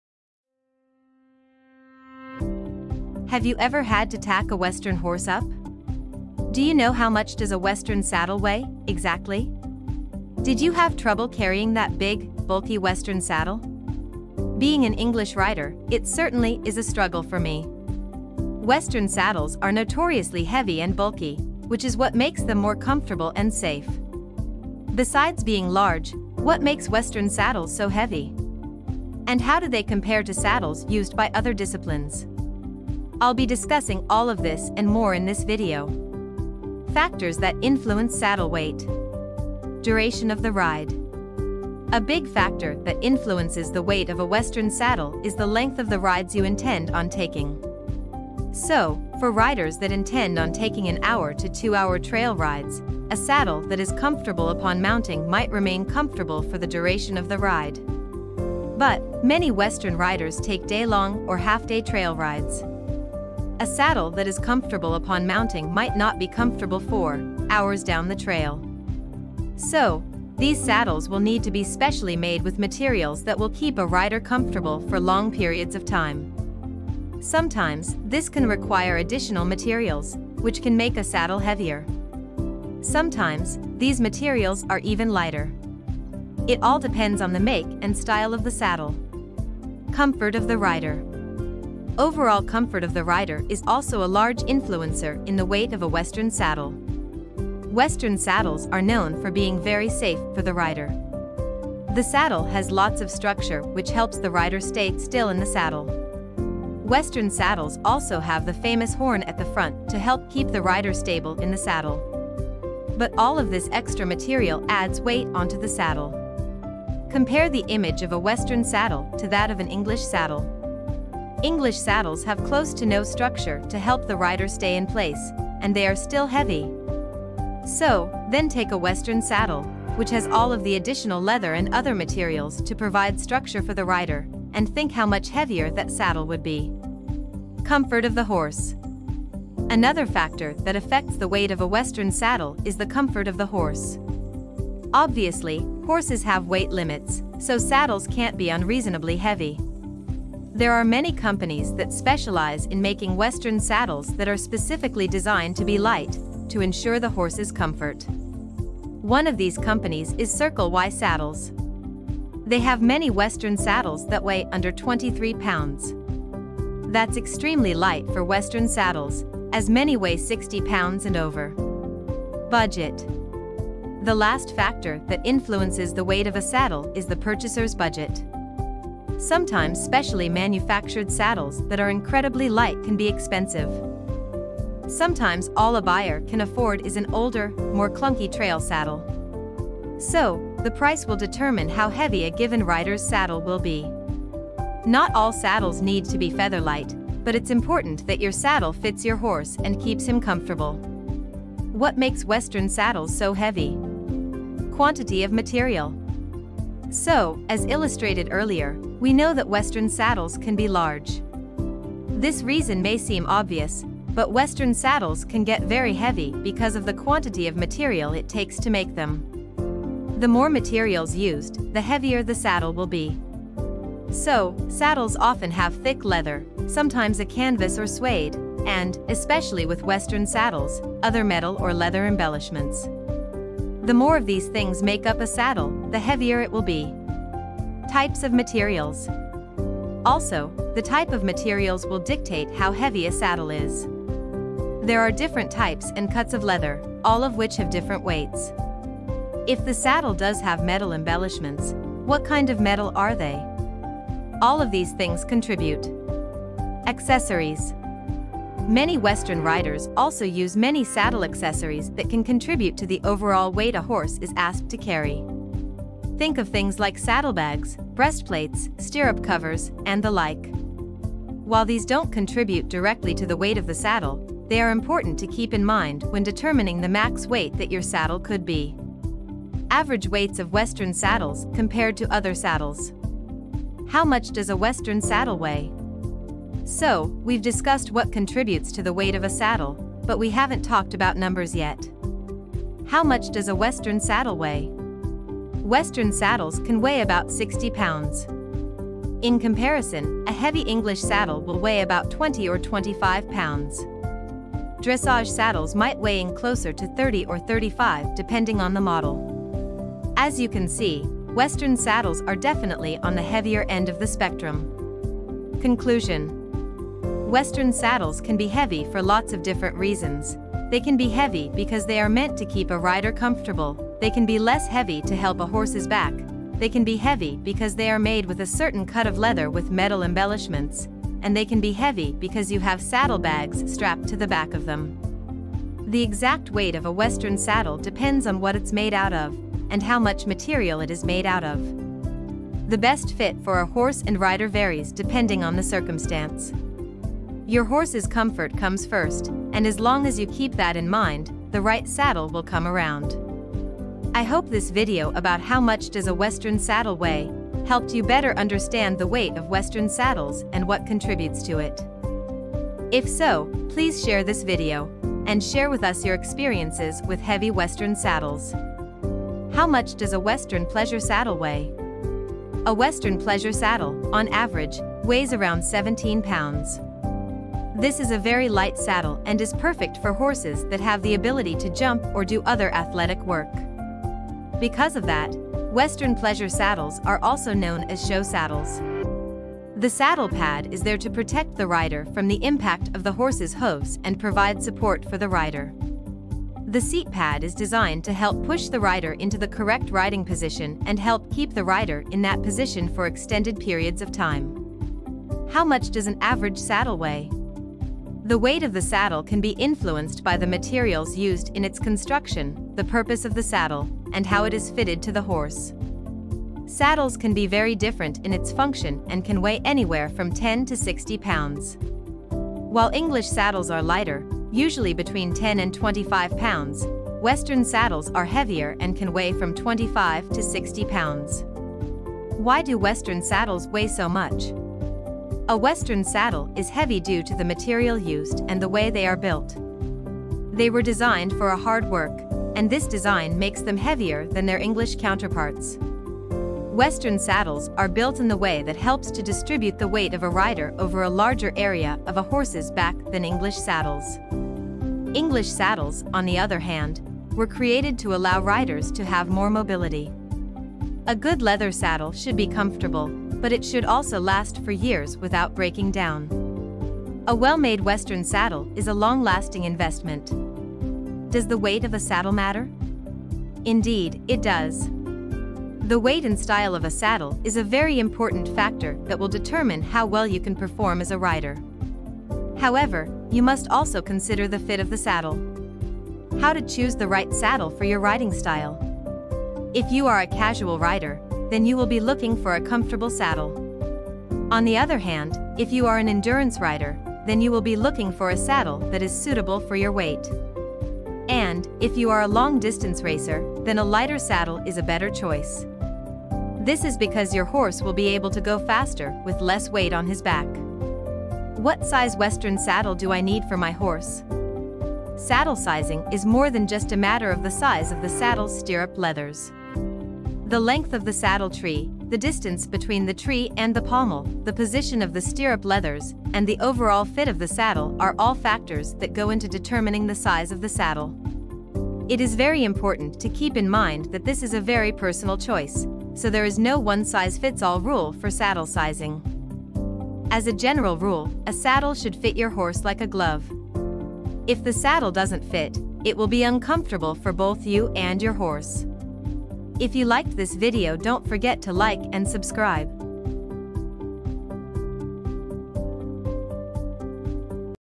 How Much Does A Western Saddle Weigh, And Why?
Have you ever had to tack a western horse up? Do you know how much does a western saddle weigh, exactly? Did you have trouble carrying that big, bulky western saddle? Being an English rider, it certainly is a struggle for me! Western saddles are notoriously heavy and bulky, which is what makes them more comfortable and safe.